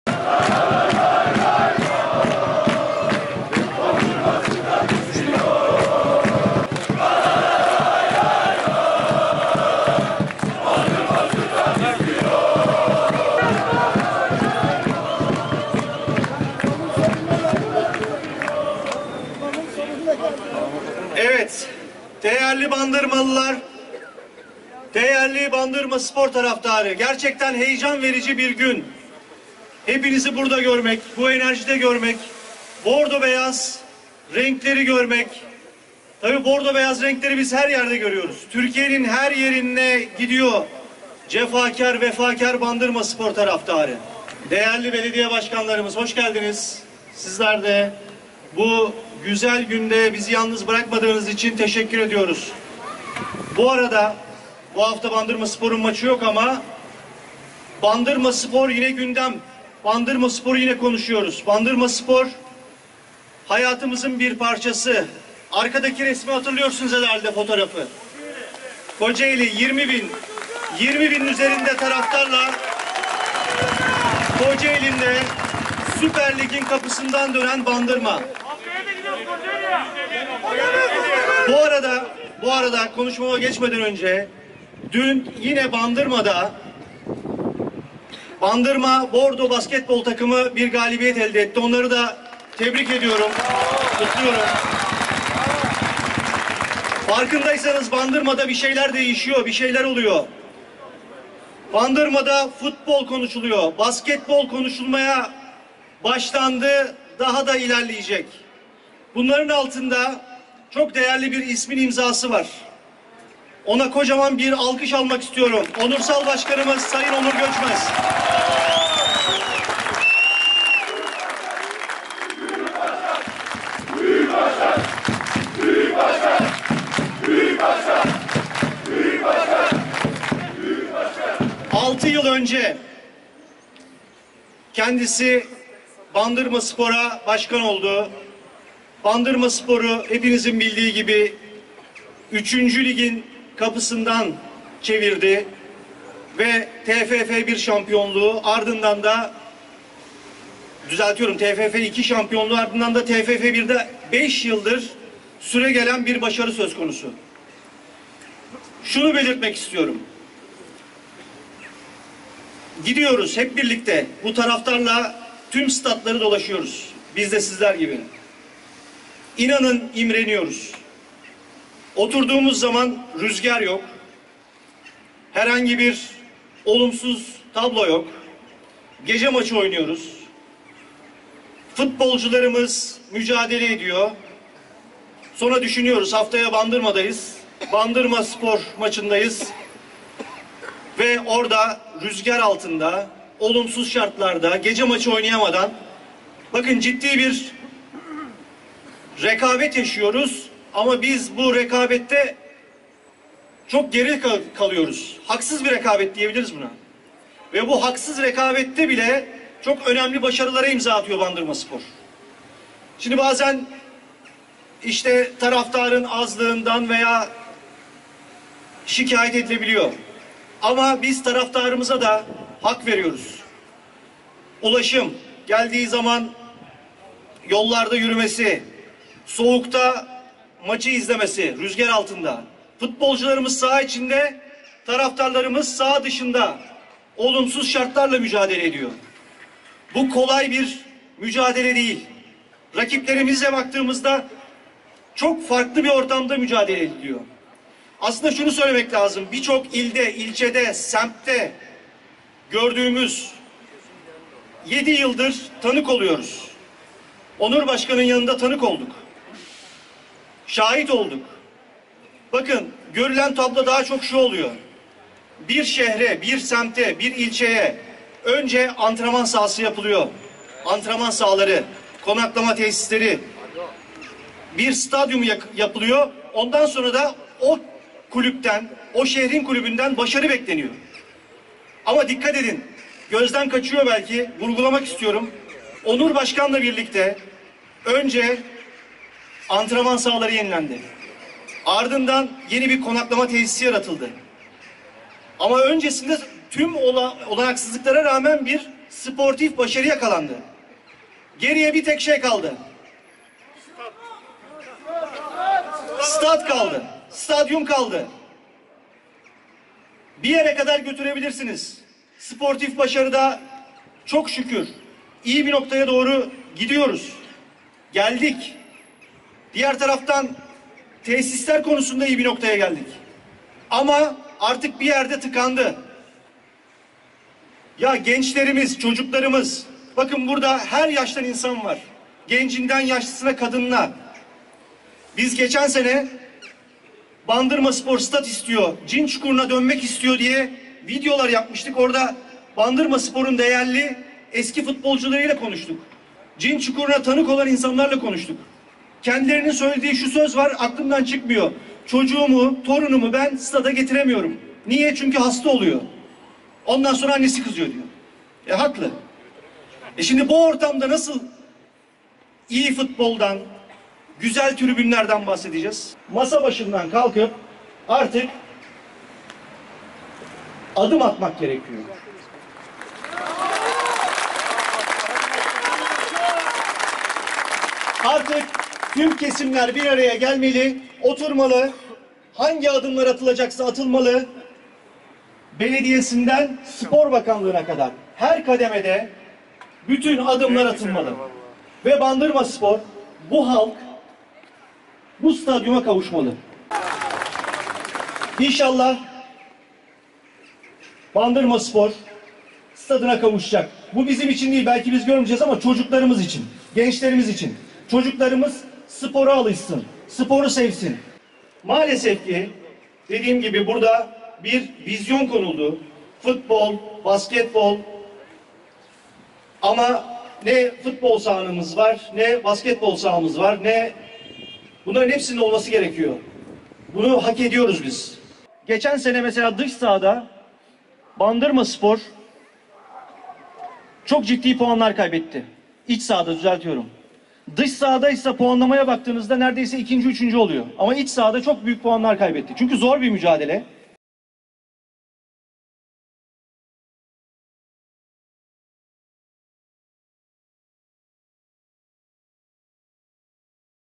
Serdar Kuter: ‘’Bandırmaspor ve bu halk stadyuma kavuşmalı’’
Bandırmaspor Kulübü Başkanı Serdar Kuter: ‘’Bandırmaspor ve bu halk stadyuma kavuşmalı’’ dedi.